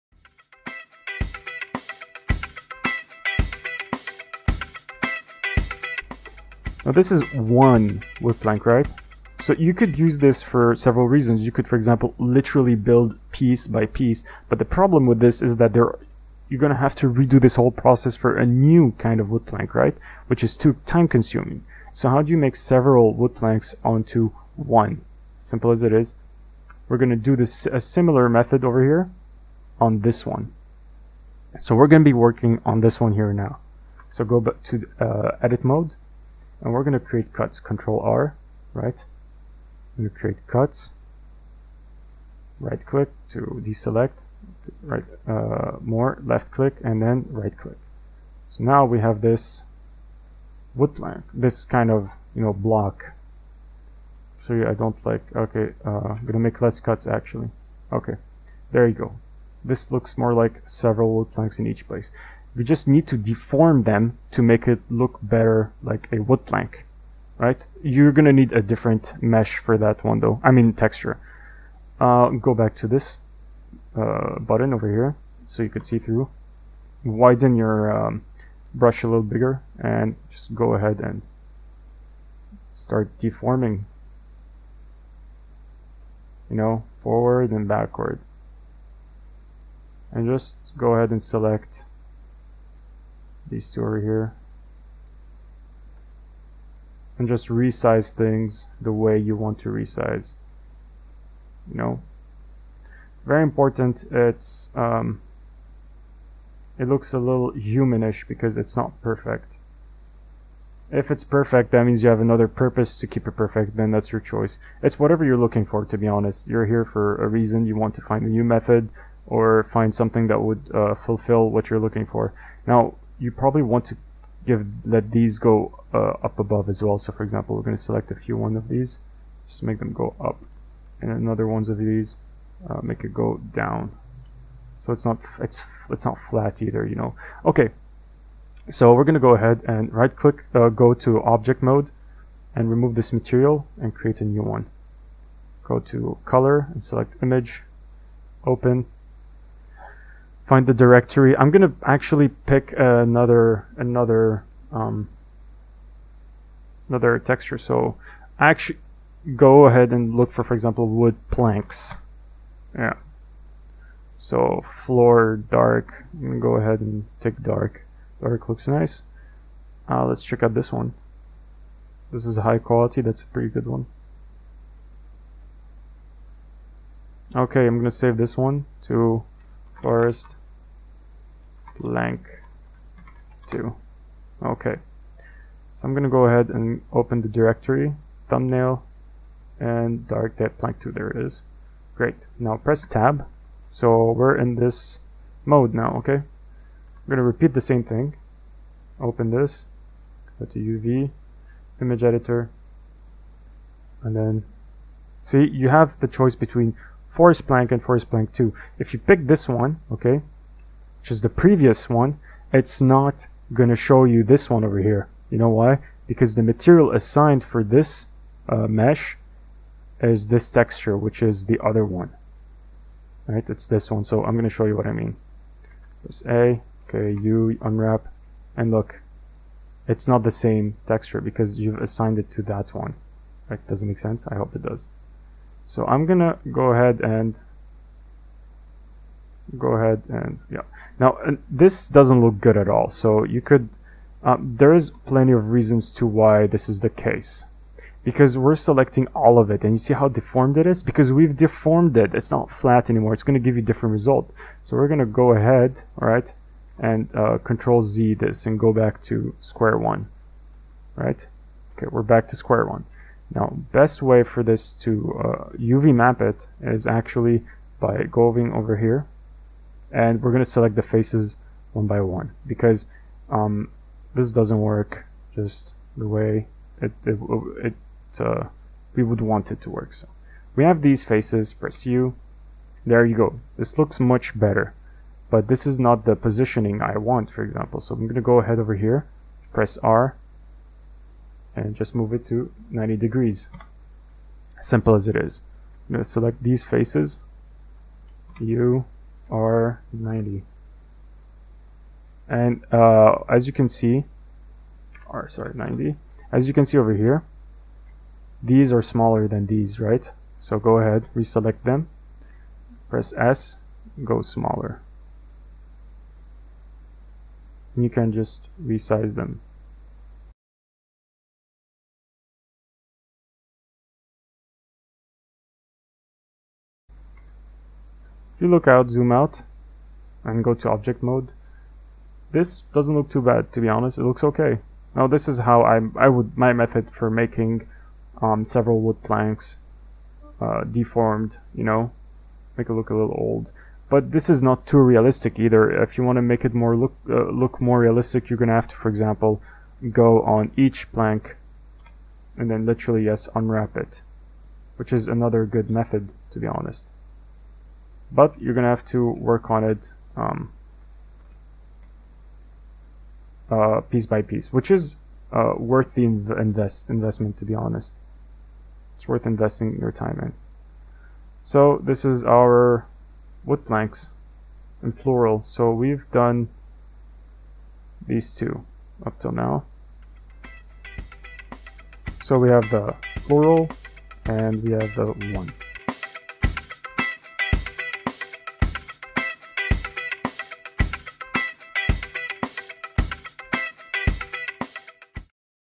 Make Wall of Wood Planks (With UV Mapping) [Blender v2.75]
In this video you'll see how to make a wall of wood planks with the same technique shown in the previous video in order to save time on larger scales.